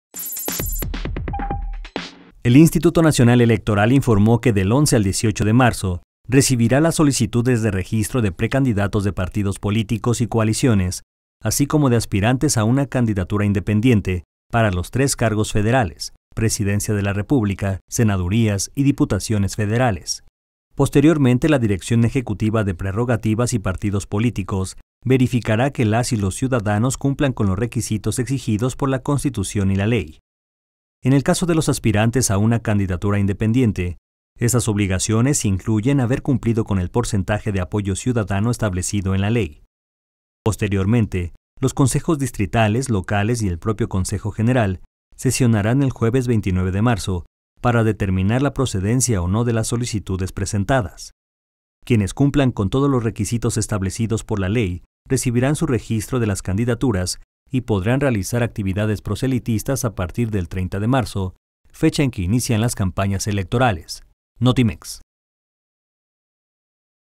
INE recibirá solicitudes de registro de candidaturas del 11 al 18 de marzo
Los aspirantes que reciban sus registros de candidaturas podrán
iniciar actividades proselitistas a partir del 30 de marzo